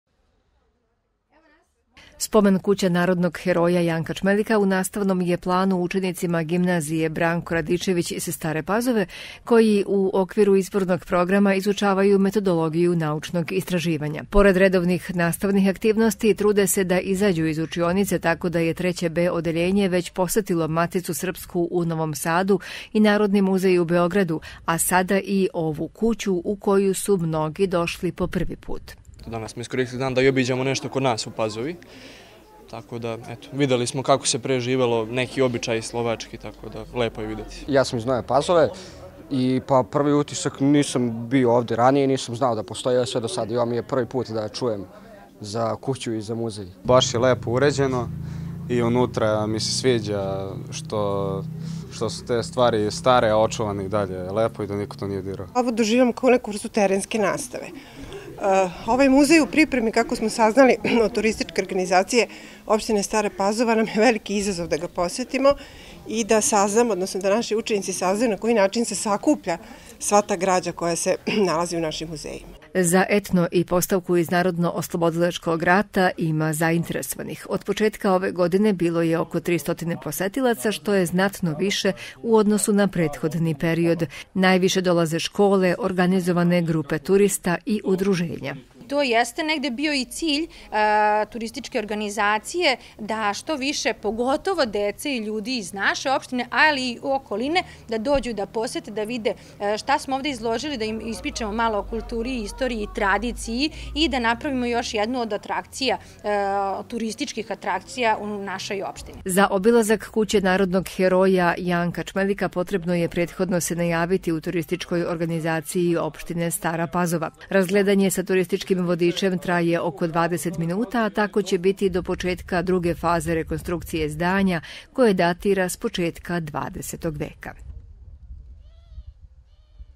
Sve više posetilaca u kući Janka Čmelika
U spomen kući narodnog heroja Janka Čmelika u Staroj Pazovi sve je više posetilaca, ali zdanje se otvara samo za najavljene posete. Zbog toga je potrebno prethodno kontaktirati Turističku organizaciju Opštine Stara Pazova, a juče su organizovano došli učenici staropazovačke gimnazije i bila je to prilika da saznaju na koji način su sakupljeni i kako se čuvaju izloženi eksponati.